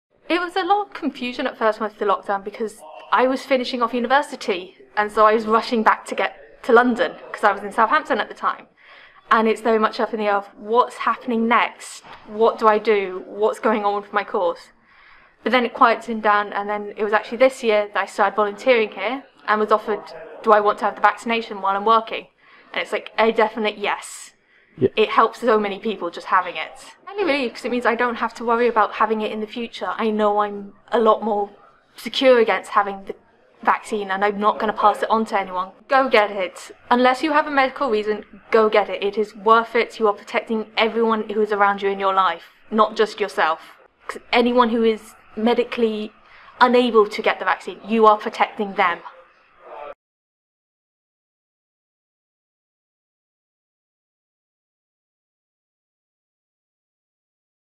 You are protecting everyone who is around you - say yes to the vaccine
Getting the vaccine is the best way of protecting yourself and others against COVID-19 and in order to encourage others to get theirs, we caught up with local people who have had or are planning to have theirs.

Eimear is a student and has had both doses of her vaccine.